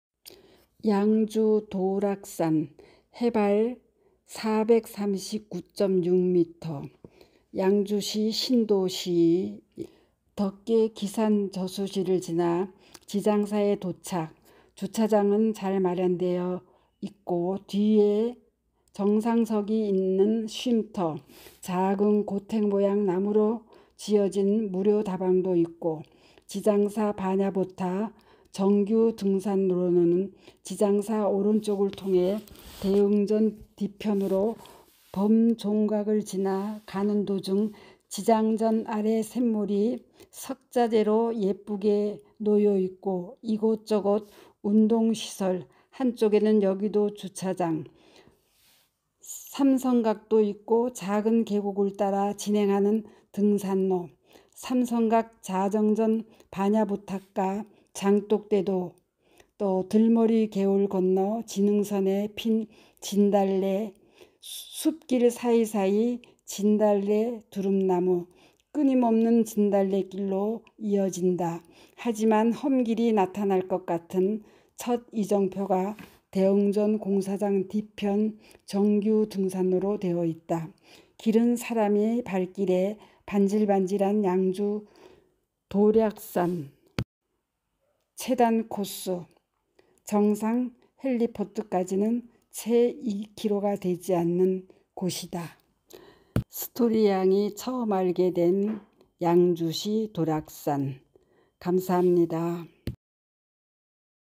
스토리양...찬조영상!양주도락산정규등산로안내? 라이딩 # 숲길 양주시 # 샘물
스토리양...양주시덕계두락산
 자전거 라이딩코스영상.
찬조 받아 알게돈 도락산 정규 등산로 .